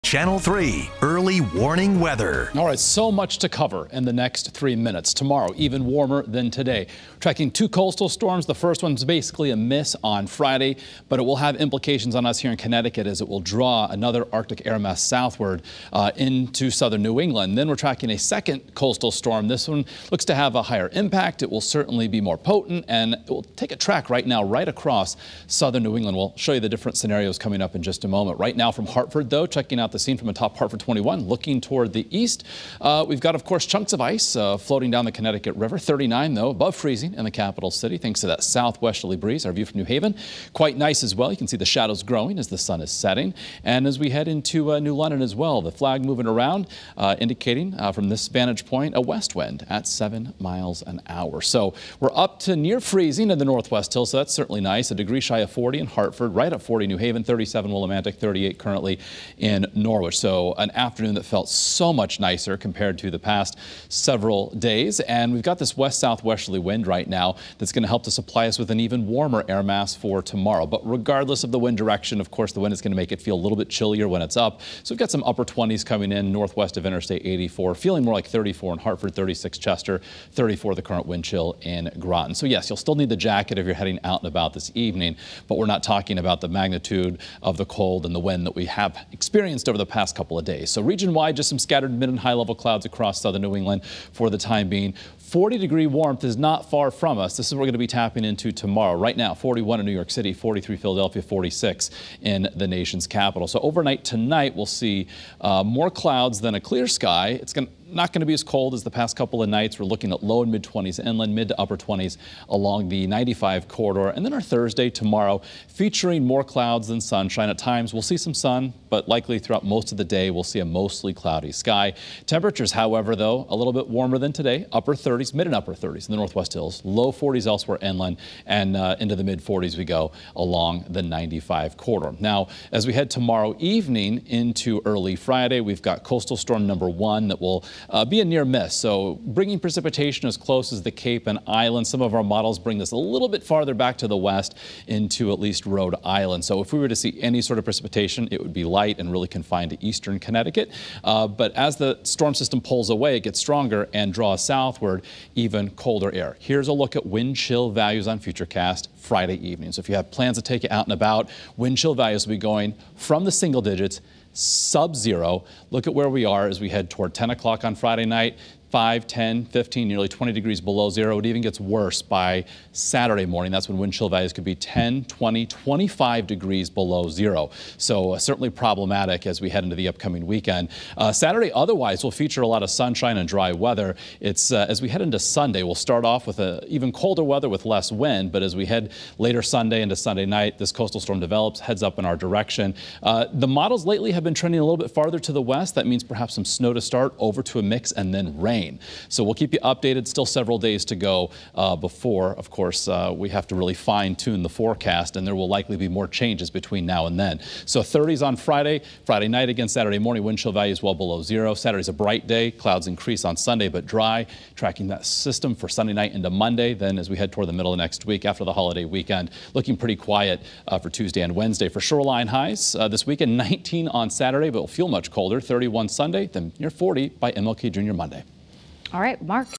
Video: Soon, temps trend milder... then more Arctic air, also a coastal storm!
Temperatures may be able to wiggle up one or two more degrees, but the winds will start to counteract the warming of the sun.